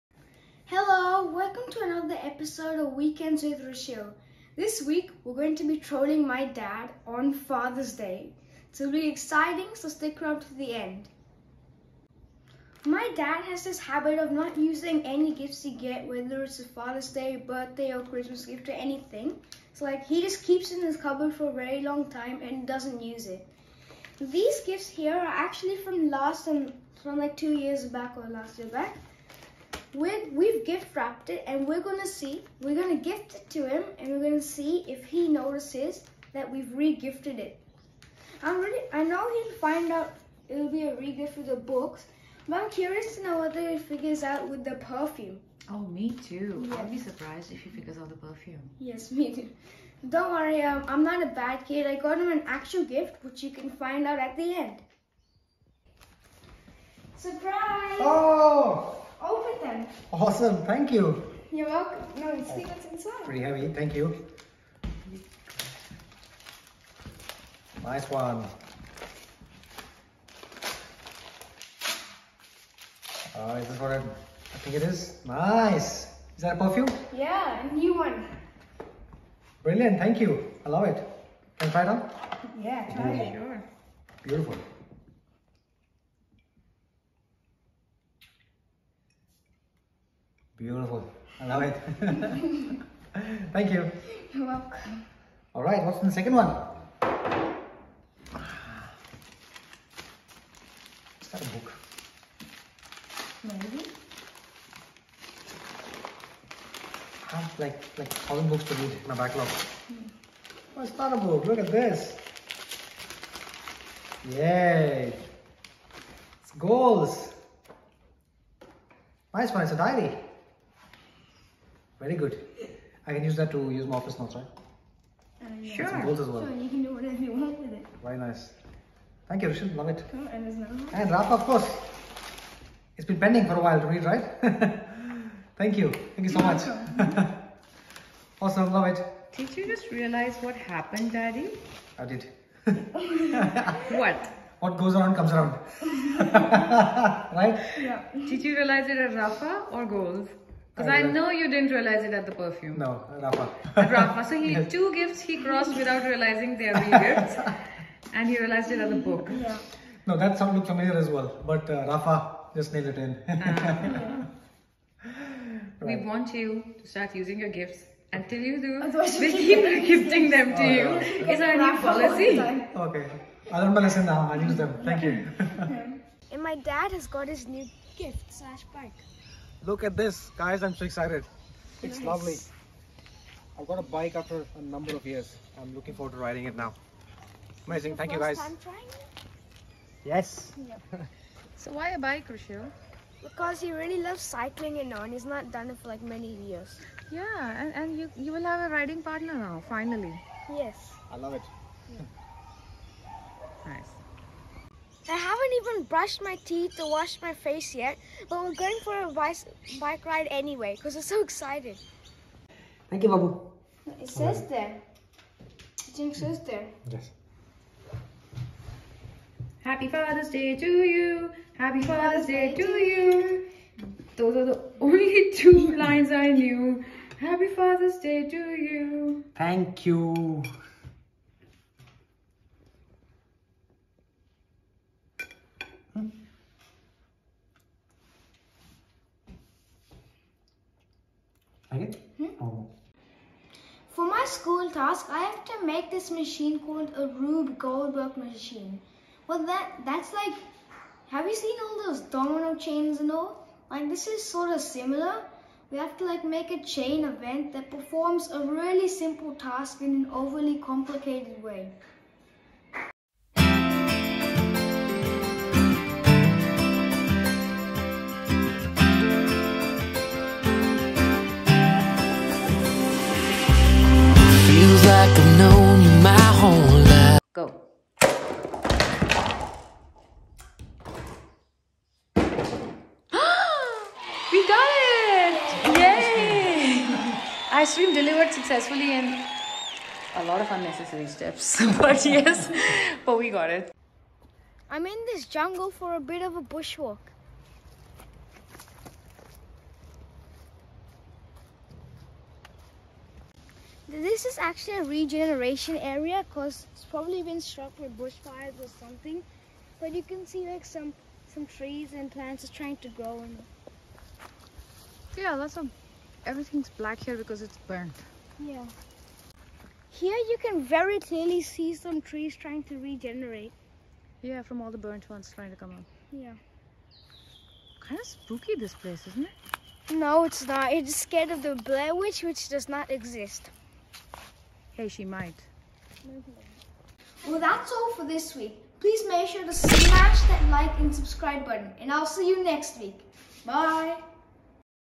Trolling my dad on father's day!!!
This weekend I celebrate father's day, bush walk and attempt a Rube Goldberg machine.